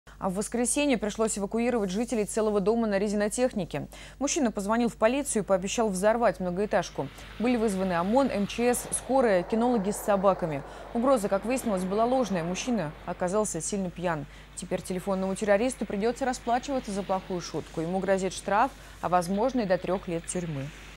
В Ярославле эвакуировали жителей многоэтажного дома
А в воскресенье пришлось эвакуировать жителей целого дома на Резинотехнике. Мужчина позвонил в полицию и пообещал взорвать многоэтажку. Были вызваны ОМОН, МЧС, скорая, кинологи с собаками.